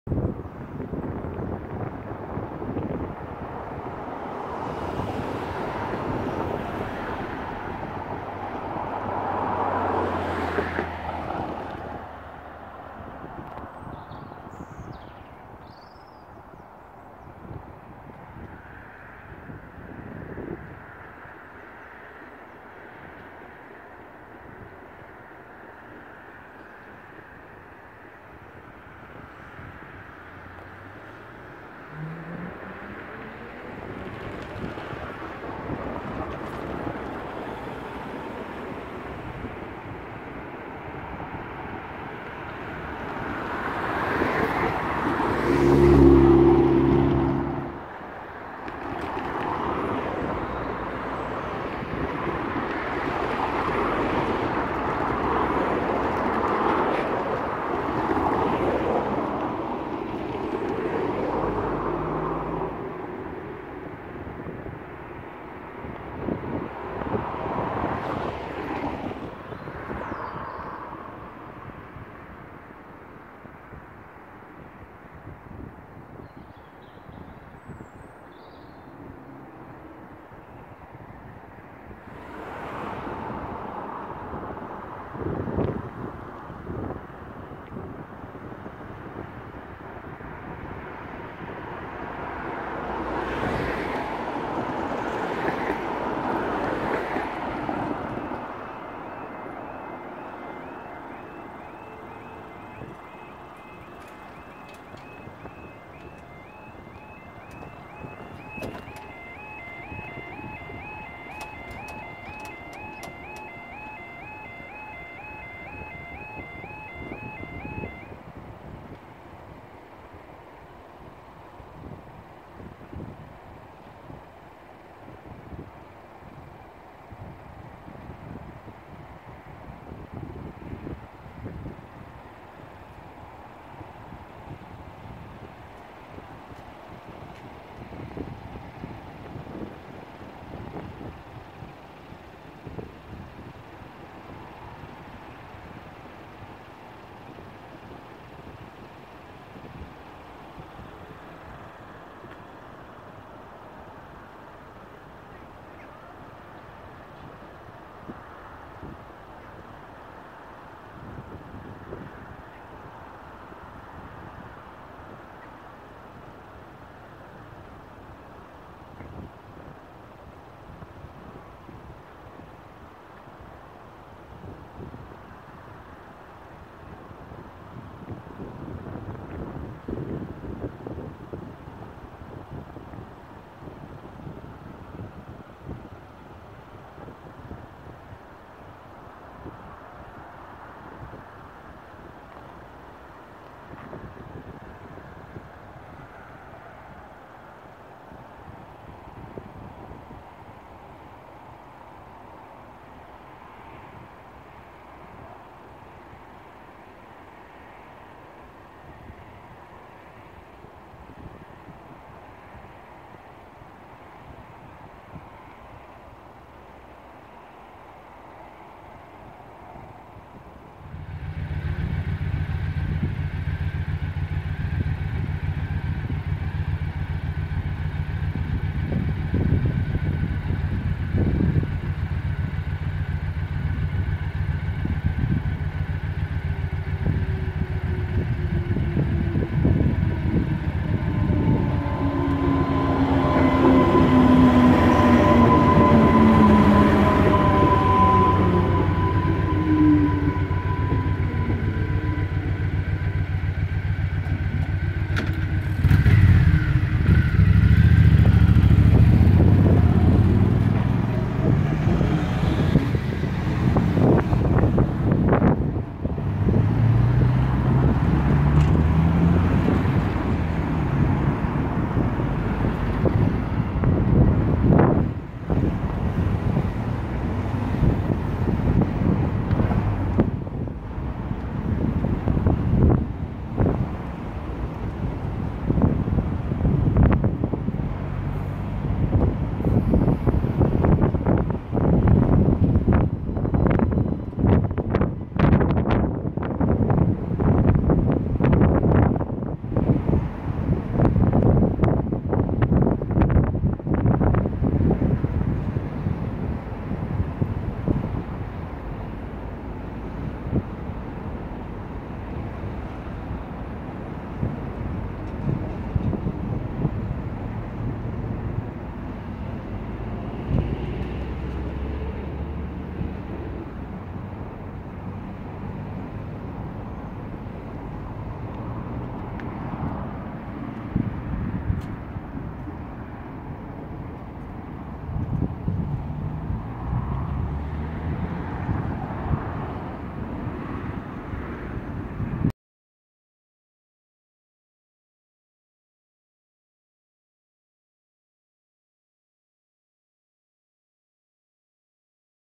Halogen lights with long shades at Bow Brickhill Level Crossing, Buckinghamshire
Route: Marston Vale Line
- Bow Brickhill Station

Location: V10 Brickhill Street, Bow Brickhill
Trains: Class 230 'D-Train' (London North Western Railway) - 3:57

Activation starts at 1:35

Level crossing found on a busy three-lane road in between the staggered platforms of Bow Brickhill Station in the south of Milton Keynes. Halogen lights still remain operational and in good working order to the present day, much like at all the level crossings on this line apart from two at the start of February 2020, with the two this side more recently modified to have long shades in 2016. Both alarms are also very quiet for the surroundings of the crossing, and this is quite a large level crossing, since the road here is three lanes wide.

A few minutes of the wait for the train has been cut out since the train took around 6 minutes to arrive after the sequence started, causing traffic chaos as the barriers were lowered for an excessive amount of time just for a single train. The D-Stock Class 230 can be seen departing the station for Bedford at the end of the video.